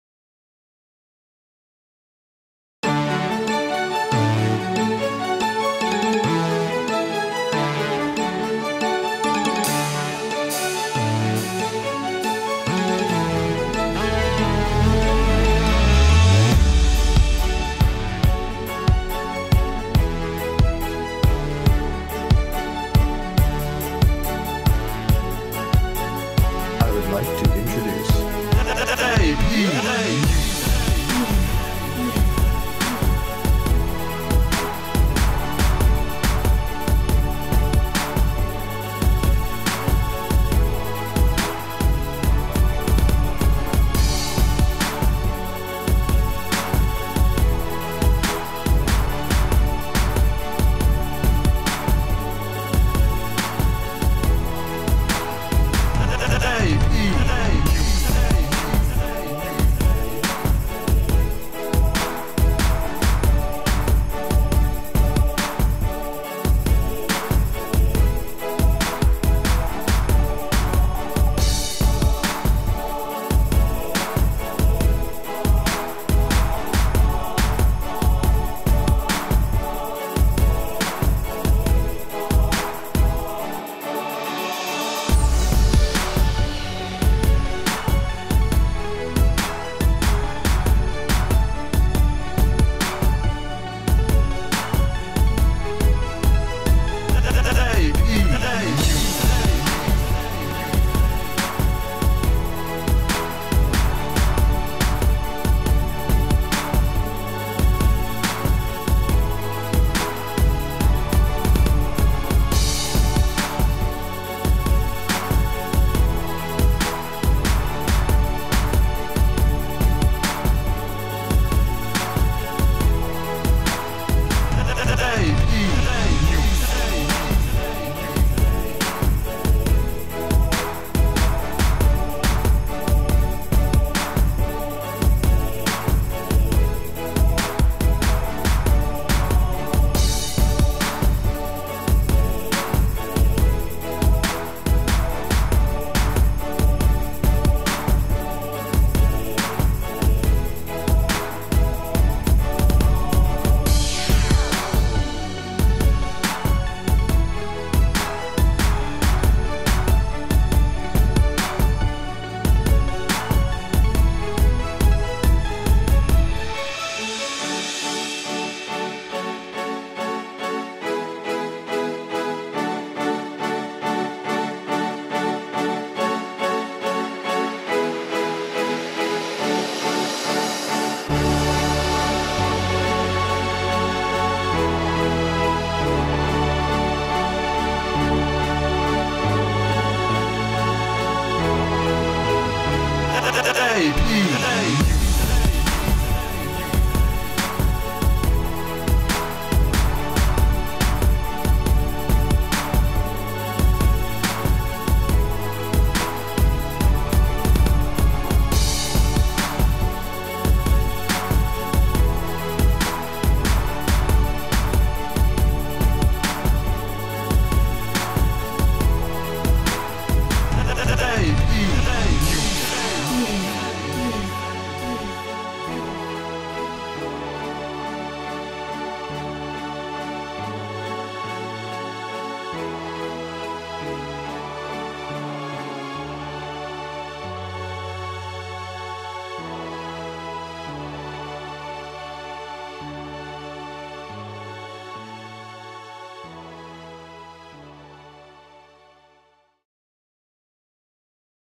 Fl Studio 11 Uplifting Hip Hop/ Rap/ Choir Instrumental
New beat created by me in FL Studio 11 for Uplifting Rap or Hip Hop. Let me know what you guys think in the comment section.